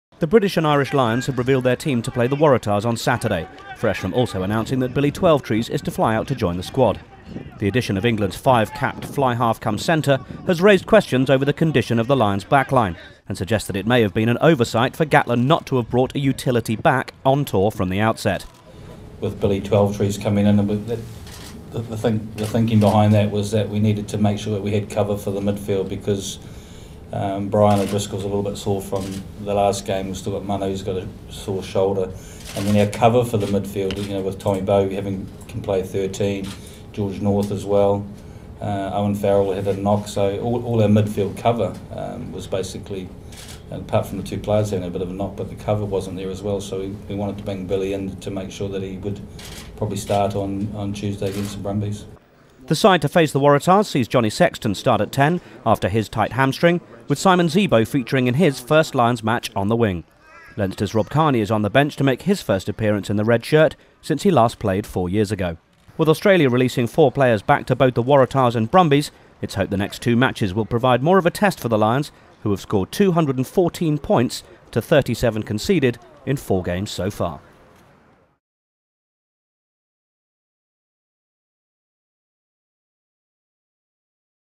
Lions 2013 - Twelvetrees called up as Lions name team to face Waratahs
The Lions will play The Waratahs in Sydney on Saturday and Warren Gatland has named his team to face them following news that England's Billy Twelvetrees is to join up with the Lions squad.